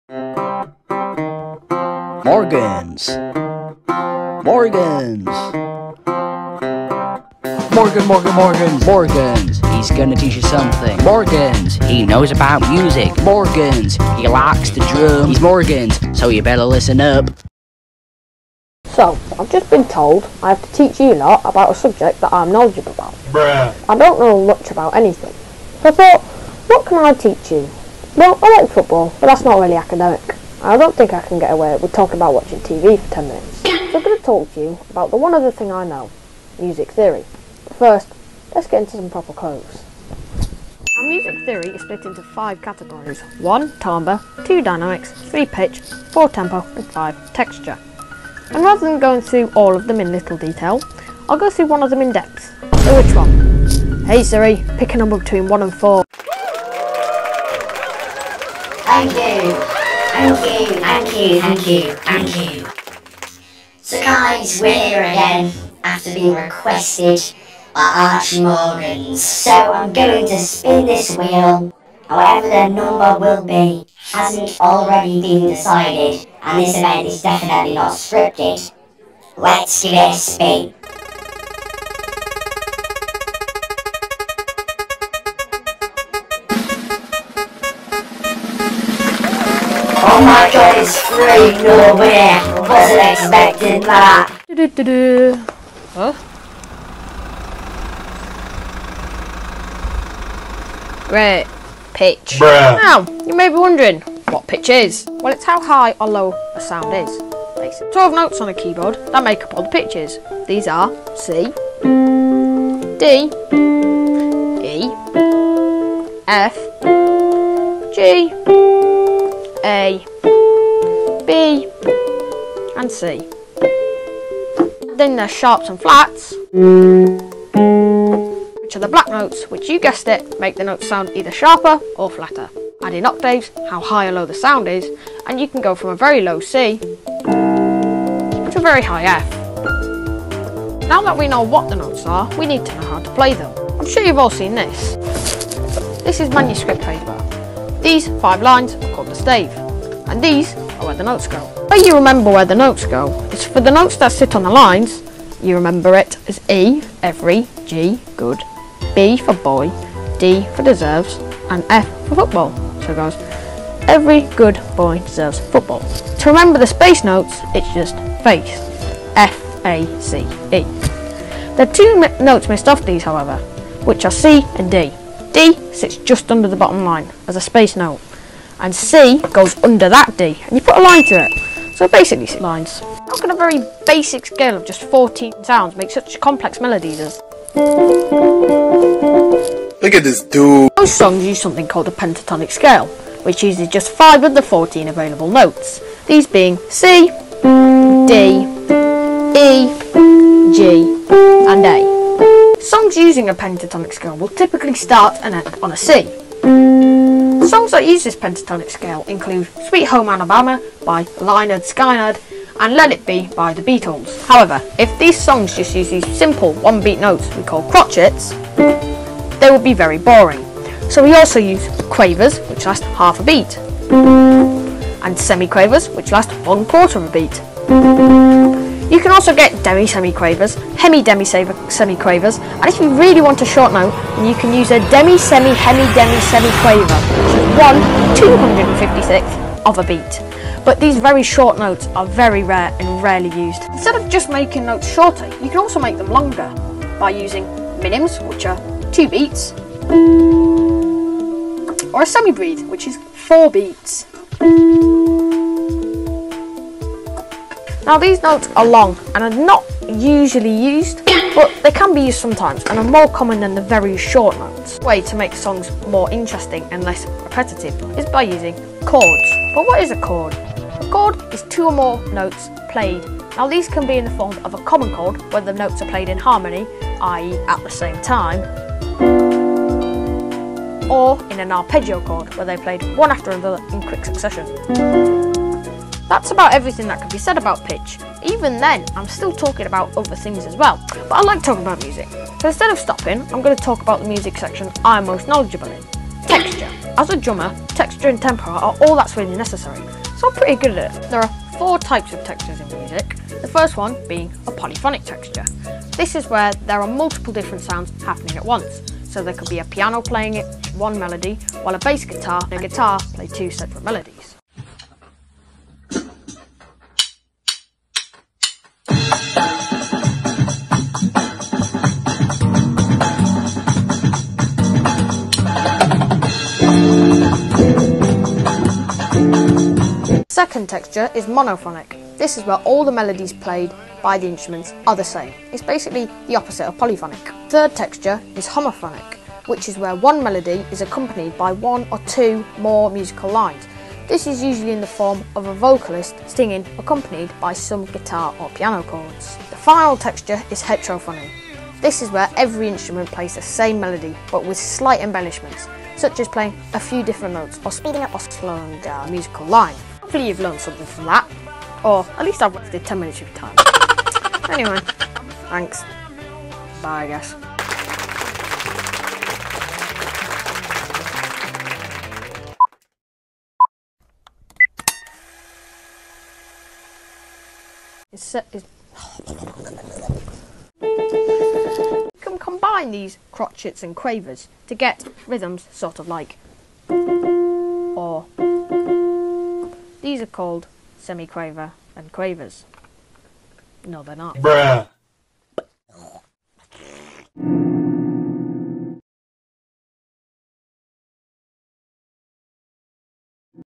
Archie Morgans Teaches You About Music | Scholar Program
This is a video that I edited for my friend Archie Morgans that he used for his scholar program.